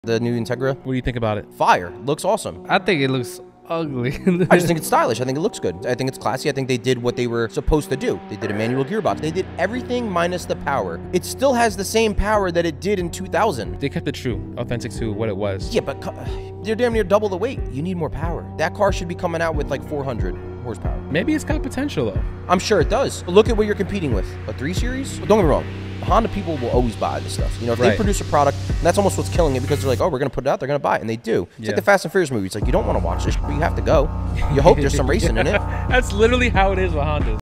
The New Integra Type S Fails To Impress Honda Owners 😩
​ Frankie Type R on why the new Integra Type S isn't the same as it used to be